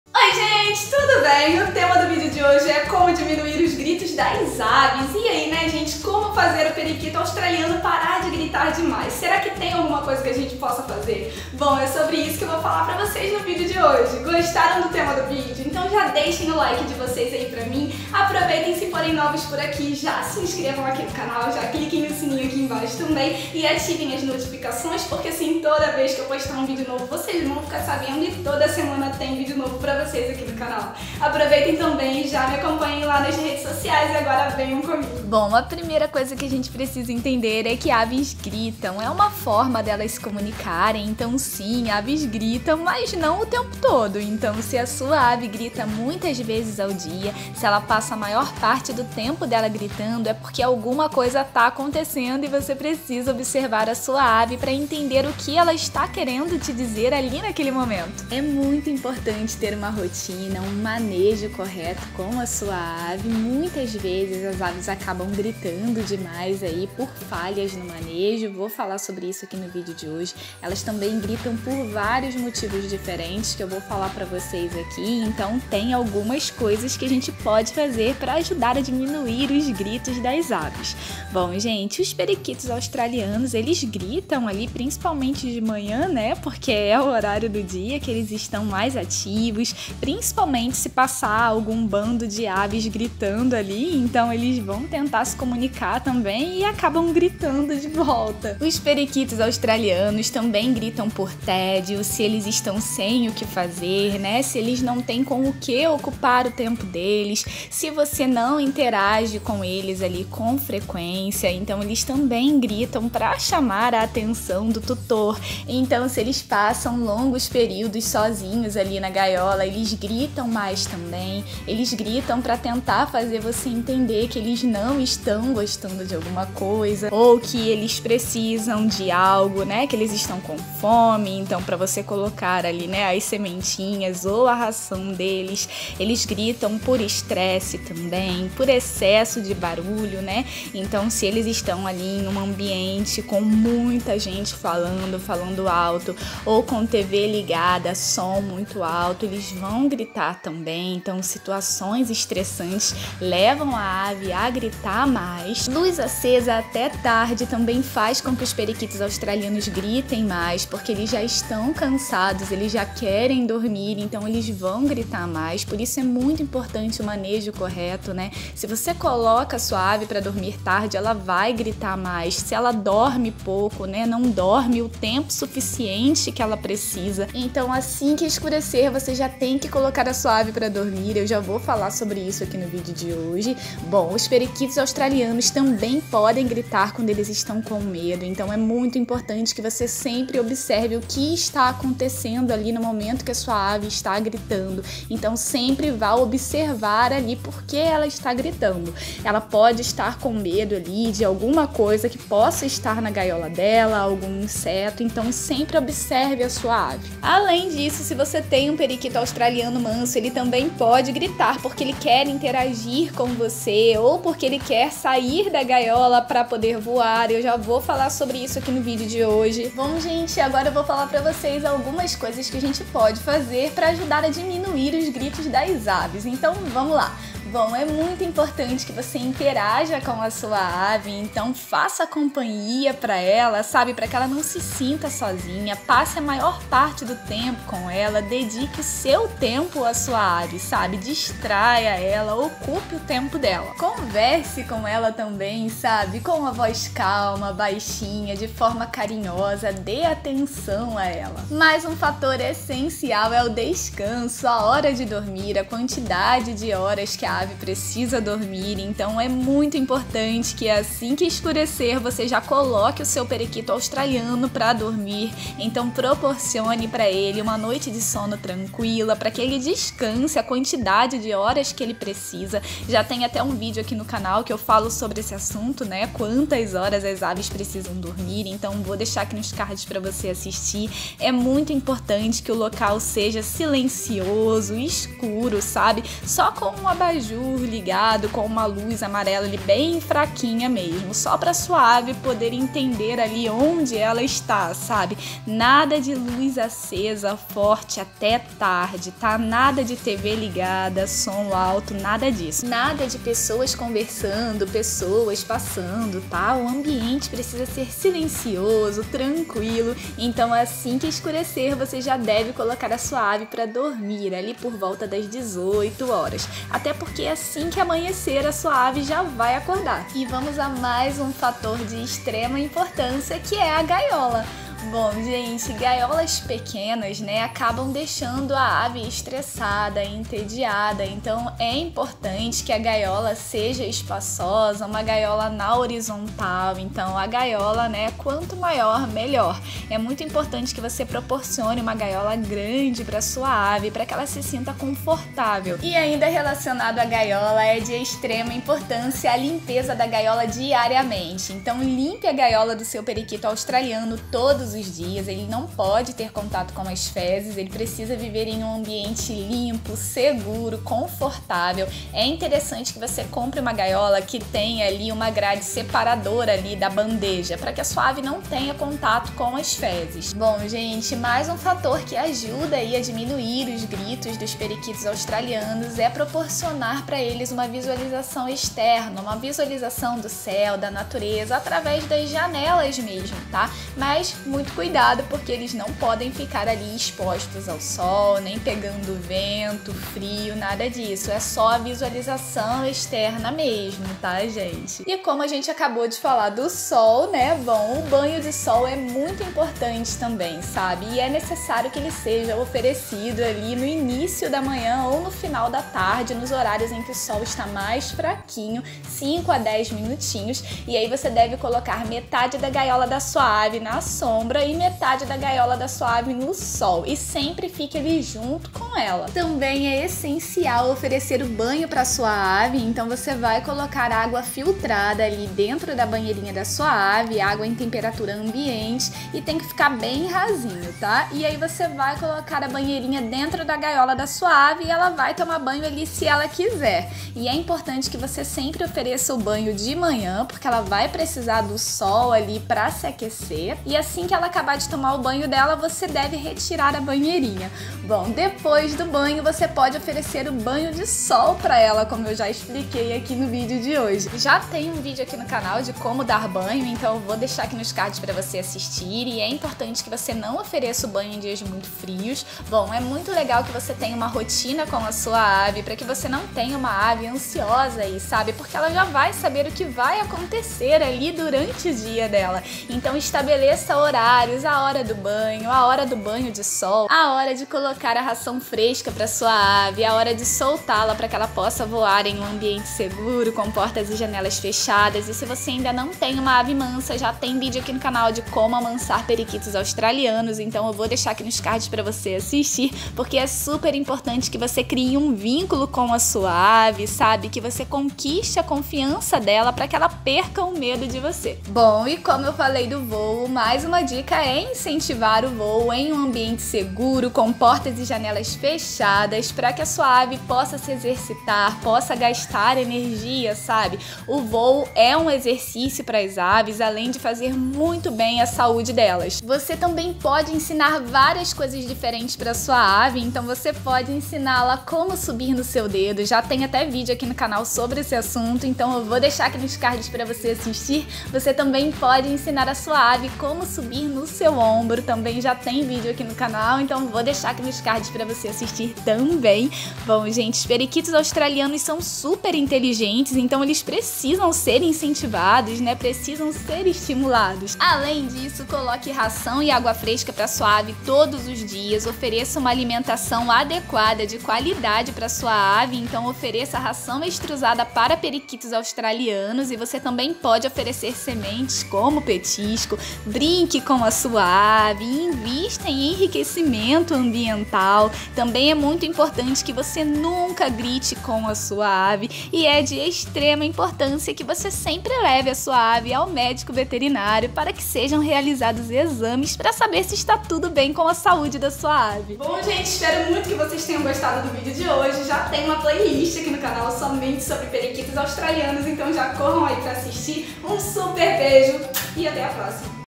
COMO FAZER PARAR DE GRITAR DEMAIS - COMO DIMINUIR OS GRITOS DAS AVES - Periquitos Australianos
Oi meus amores, o vídeo de hoje é sobre o grito das aves, o grito dos periquitos australianos. Você tem dúvida se periquitos australianos gritam, vocalizam muito?
Já se perguntou se tem o que fazer para sua ave parar de gritar demais, como fazer seu periquito australiano parar de gritar demais?
Como diminuir os gritos das aves, como diminuir os gritos do periquito australiano ou da calopsita?
Por que seu periquito australiano grita?
O que significa o grito do periquito australiano?
Por que os periquitos australianos gritam?
 Então, se você é um tutor de aves e está curioso a respeito desse assunto, fique ligadinho no vídeo.